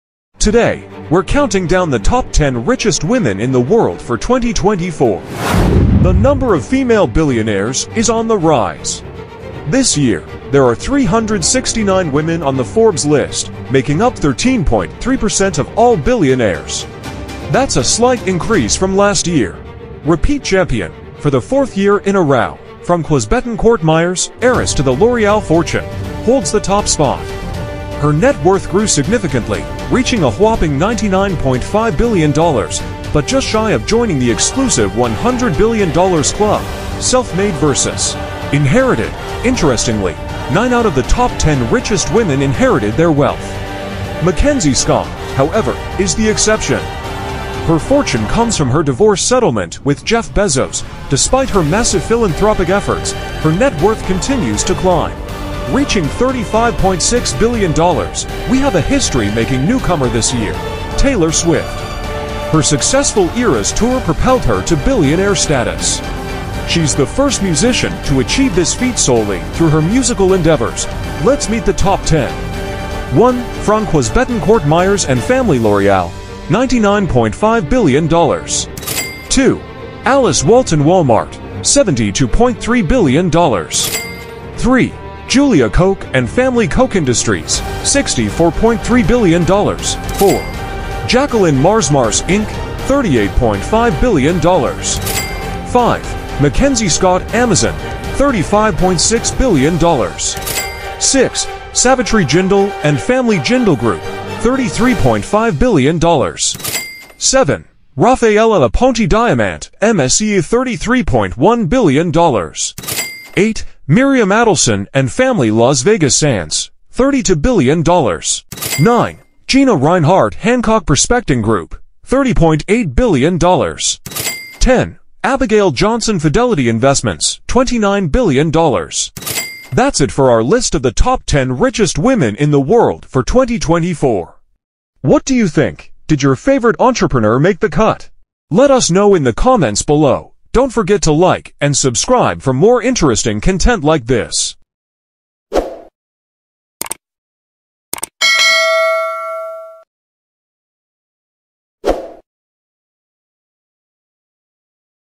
Top 10 Richest Women in The World 2024 - Forbes Latest Ranking!
Who are the richest women in the world in 2024?

From L'Oreal heiresses to self-made shipping magnates, this video reveals the inspiring journeys of the most powerful women in business.

We'll explore their incredible net worth, how they built their empires, and celebrate the rise of female billionaires!